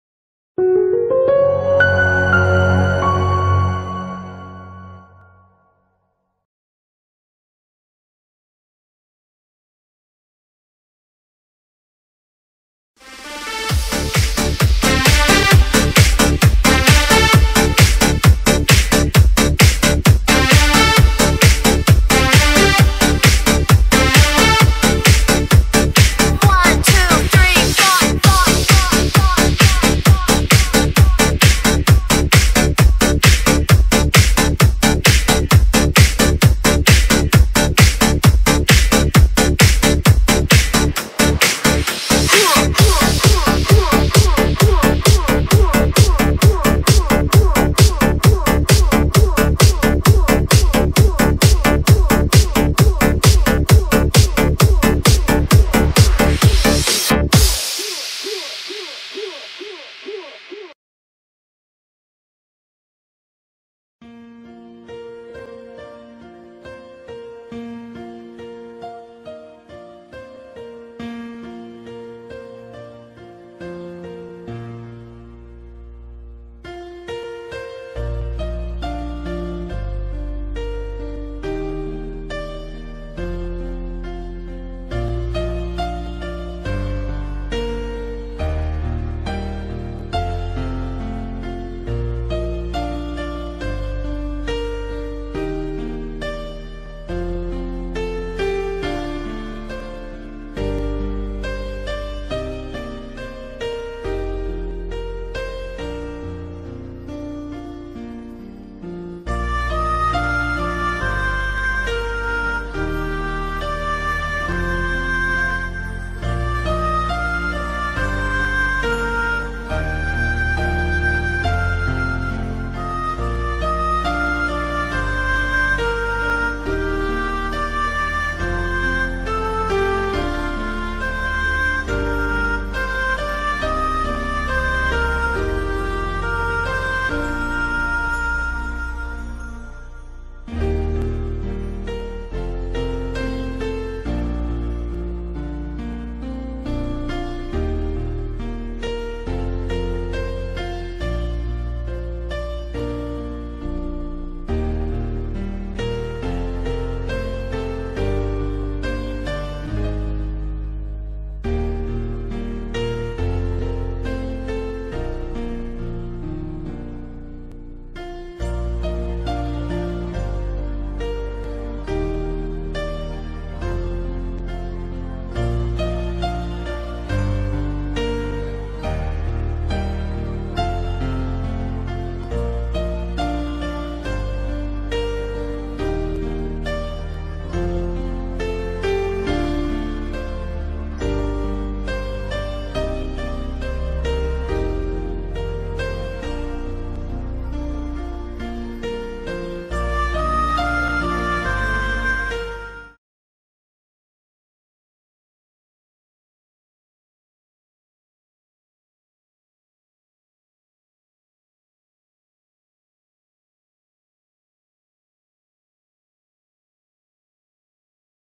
LED Infinite 3D Dance Floor
LED infinite 3D dance floor use the two lays temper glass to appear the infinite and reflect and expand more space, the multiple dimensional and bright light match up with the entertainment design, mutual complementation.